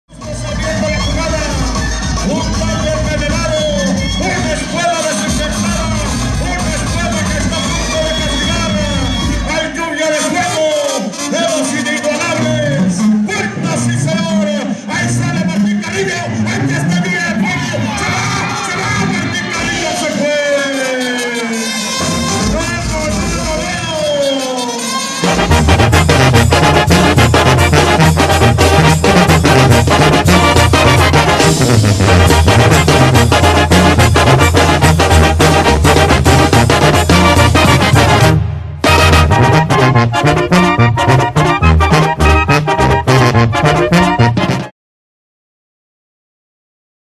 LA LLUVIA DE FUEGO UN TORO ESPECTACULAR EN COLIMA
LLUVIA DE FUEGO CONTRA MARTIN CARRILLO DE COLIMA, ASI FUERON LAS COSAS EN LA PLAZA DE TOROS DE ALMOLOYAN COLIMA